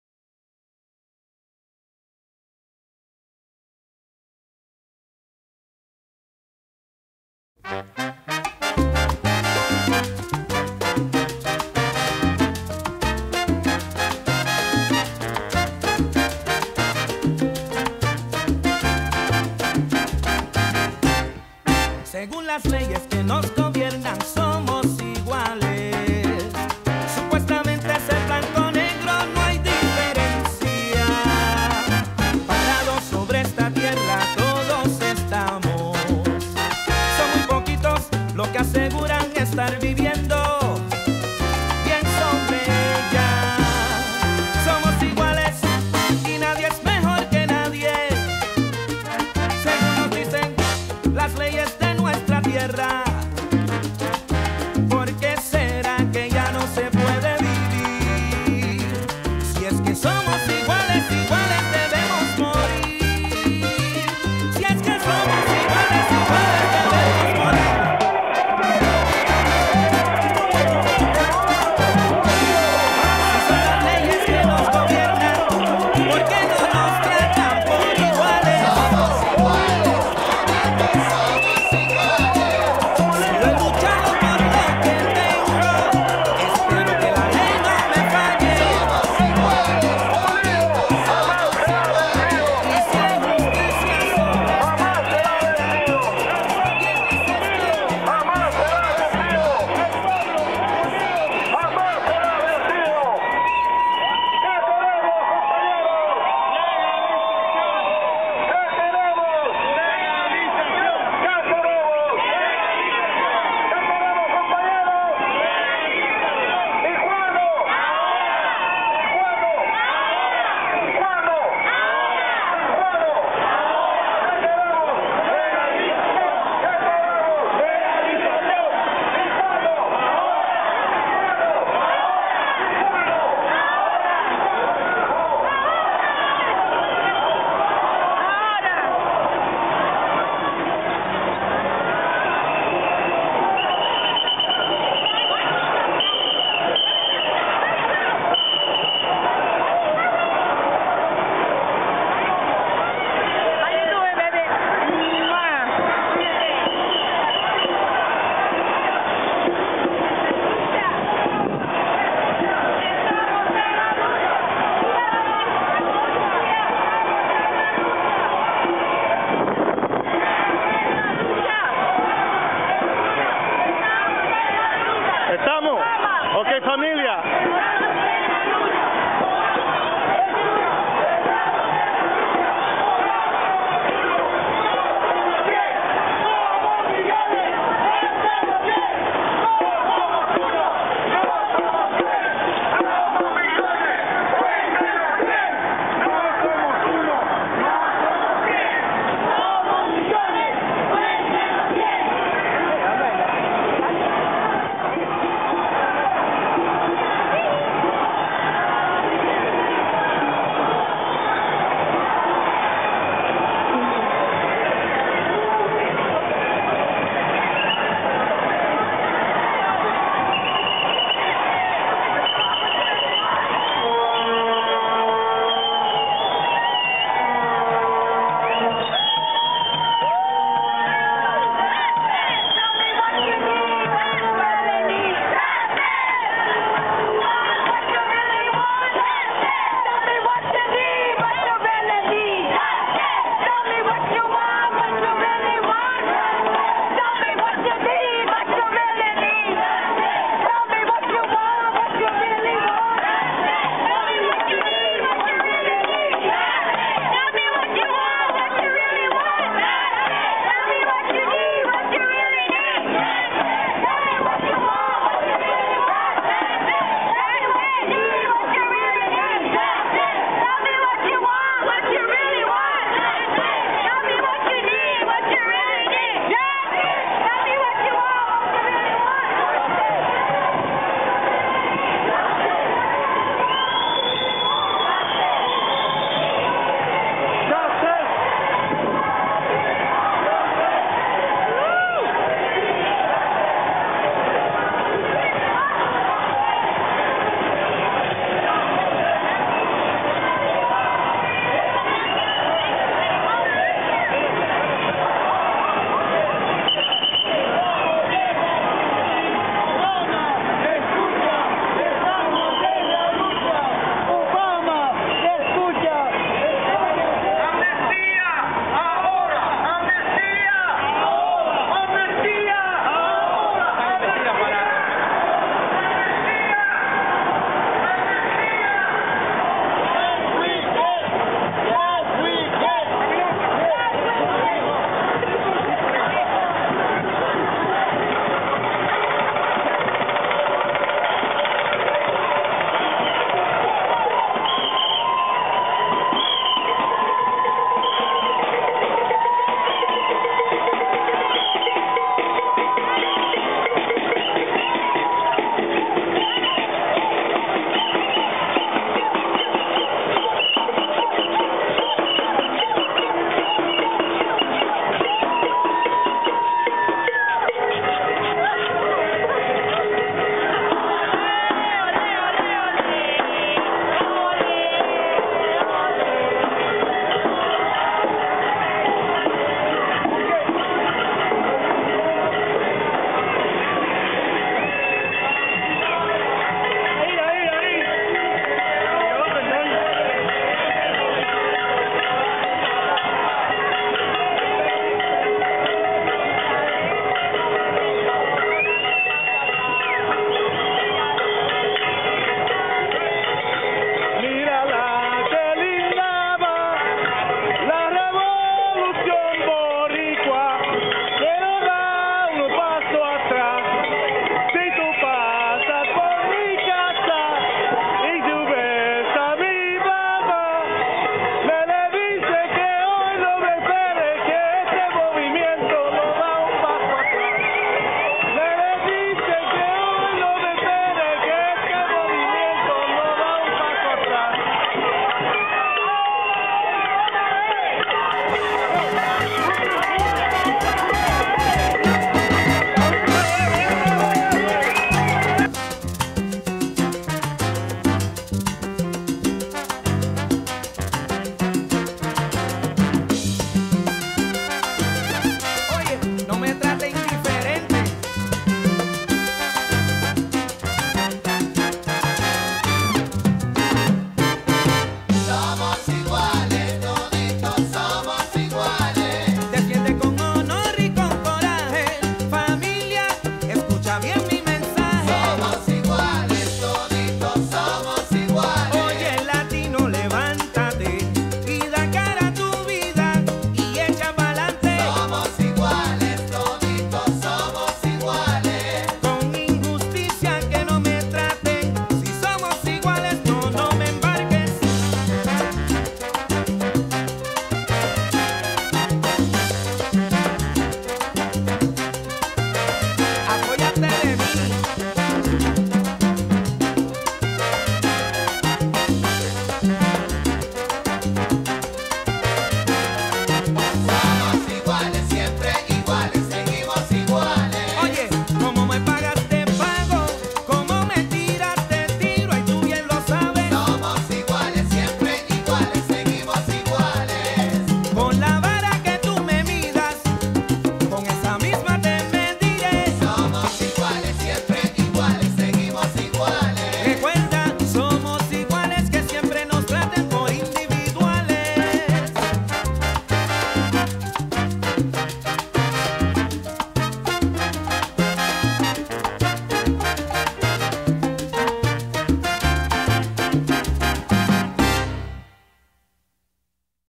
May Day!
May Day in NYC 2010 the Rally and March started at Union Square Park and made its way down Broadway. Thousands took to the streets for human rights.